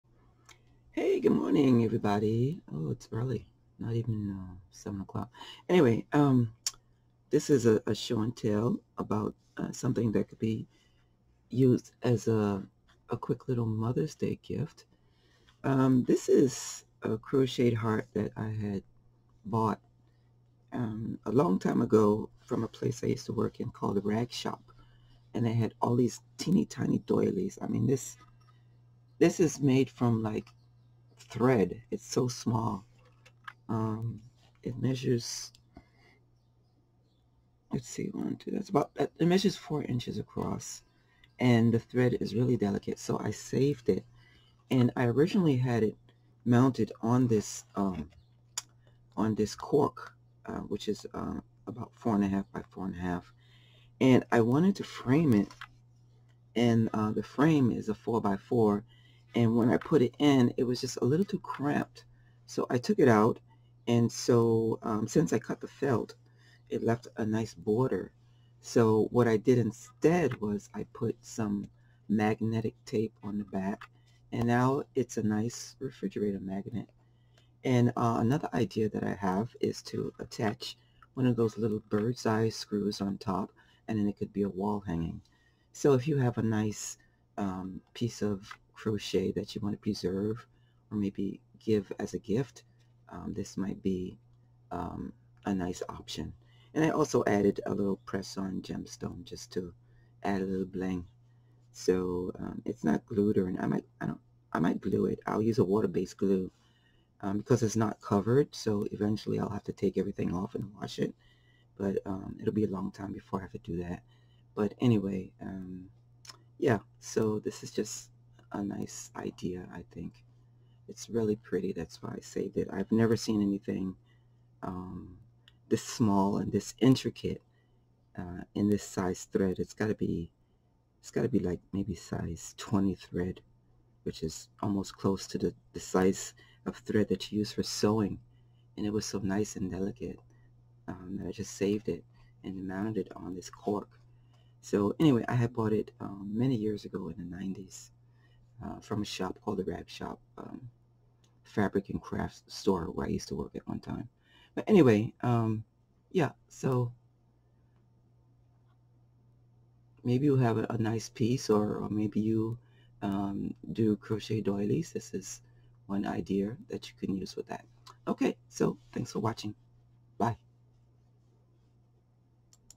Crochet: Doiley Refrigerator magnet, or wall hanging
A show and tell video about how to use a doiley into a refrigerator magnet, or wall hanging.